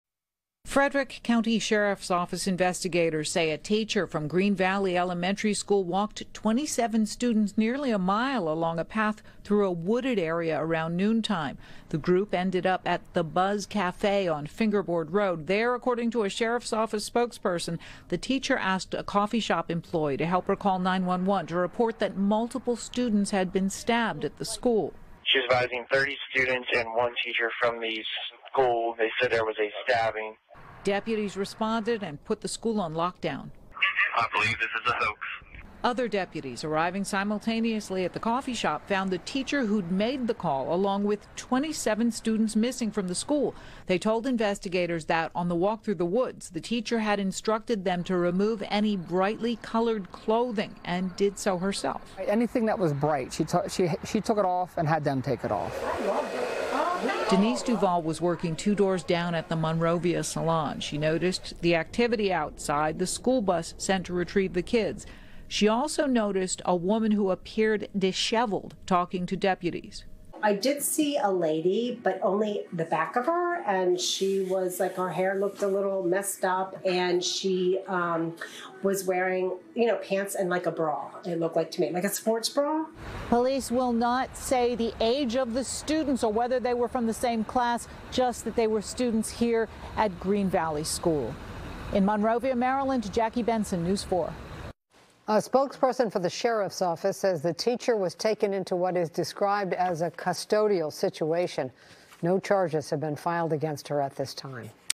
Frederick County Sheriff Investigates Unusual Incident at School | NBC4 Washington
A teacher took students from an elementary school to a café and reported multiple stabbings that didn’t happen, the sheriff said. News4’s Jackie Bensen reports.